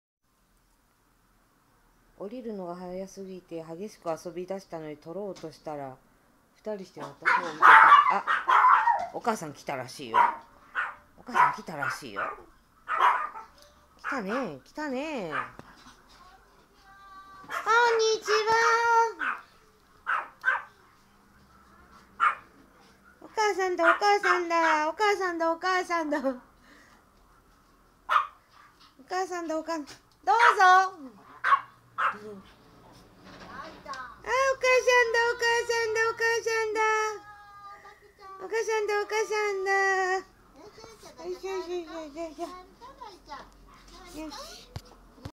野海棠1世大蔵+開ヶ実2世+権兵衛蜜柑1世

南京海棠1世の同胎の野海棠1世(大蔵)です。
パパと買い物に来て、もっと遊ぶ、まだ帰らないとせがんで、
パパも家で仕事が待ってるので
パパのお母様のお仕事帰りに拾ってもらうということで
そのまま遊んでいました。
すっごく楽しそうだったのにお母様が迎えにきたらダッシュで
おかあさーーんって！まったくおこちゃまだねぇ～